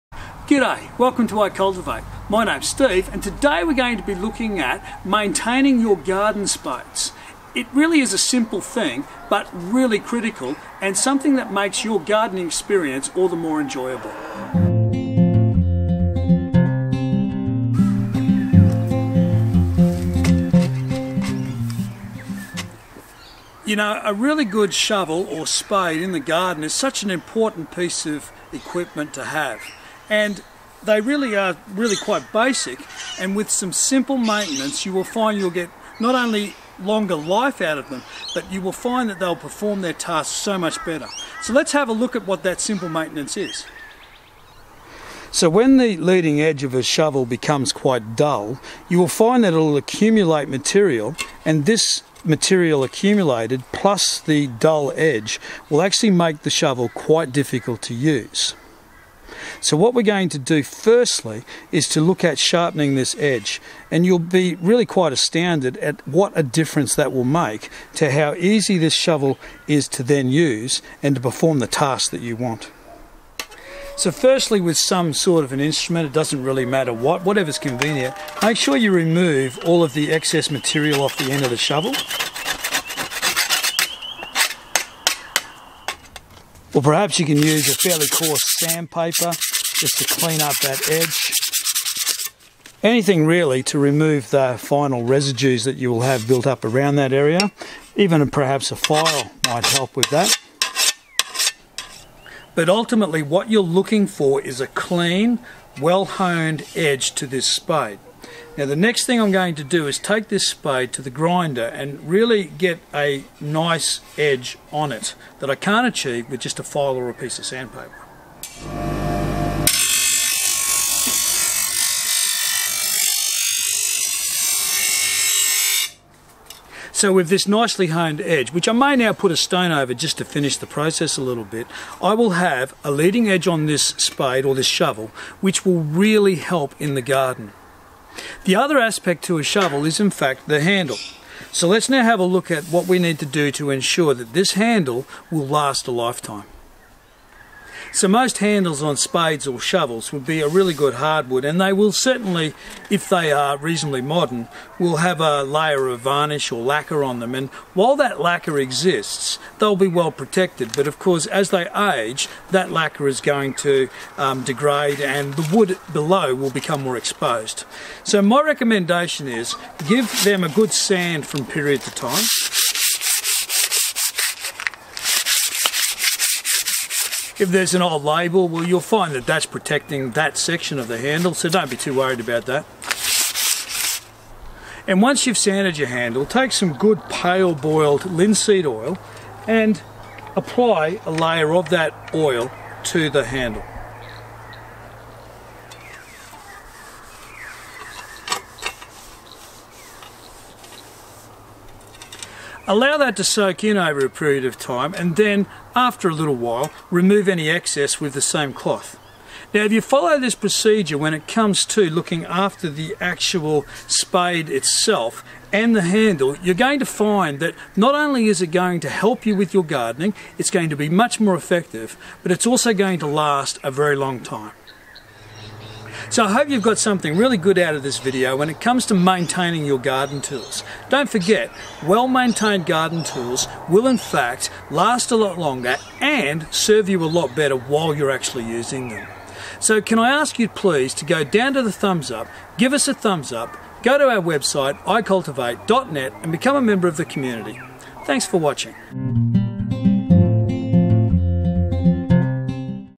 Maintaining Garden Spades
Well maintained garden tools made gardening so much more enjoyable. During this video Steve demonstrates how to keep your garden spades in top well maintained condition.